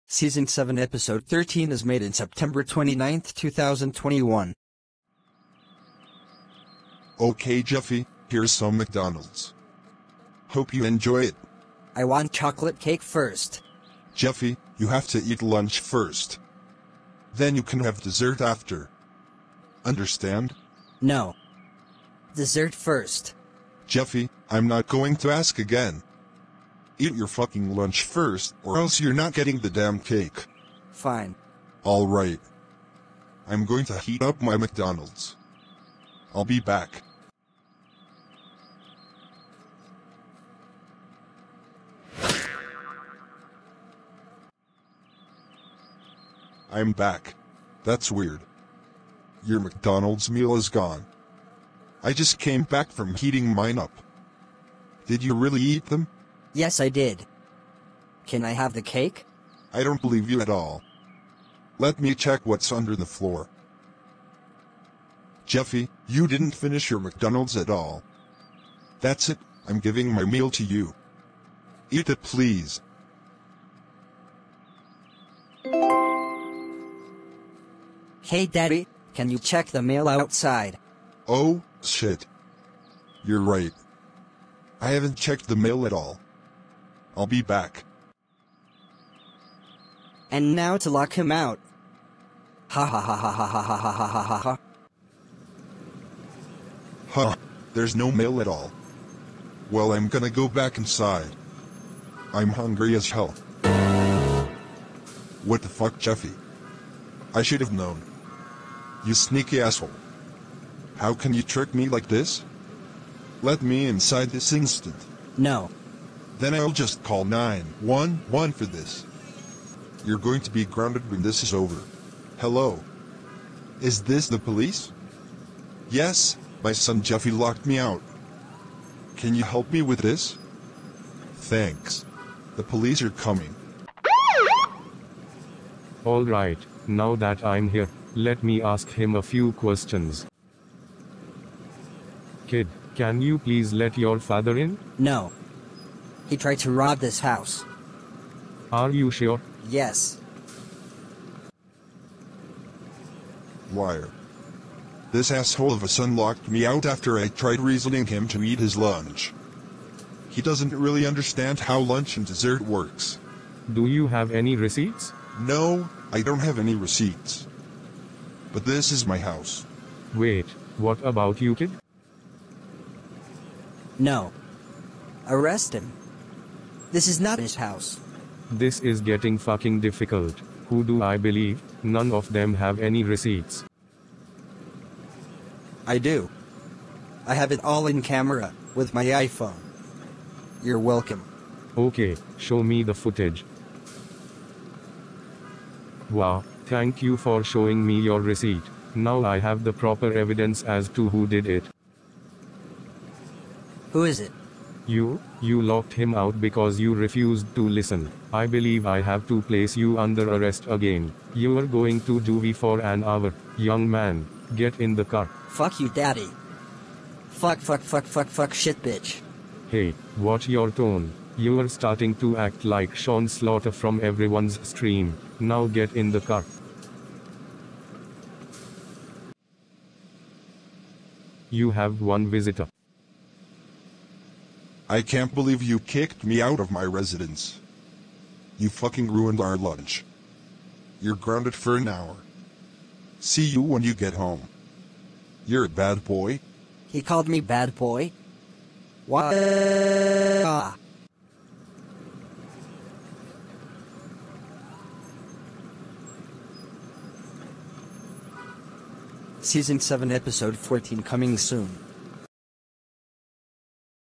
Space Adventures S7E13 - Locked Out Parody
This is how I would rewrite SML Movie - Locked Out

GO BANANAS!!!